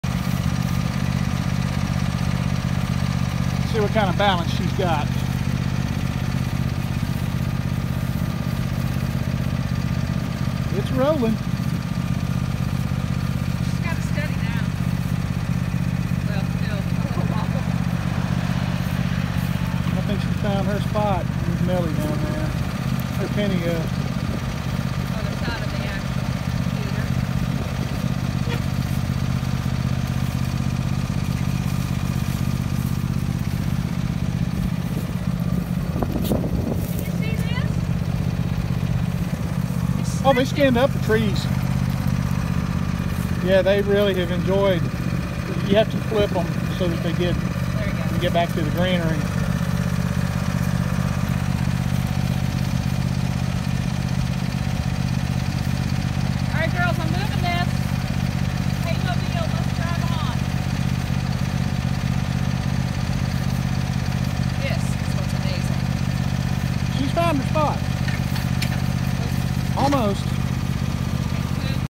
Hay-mobile part I
The goats couldn’t wait for the feeder to be filled so they hitched a ride.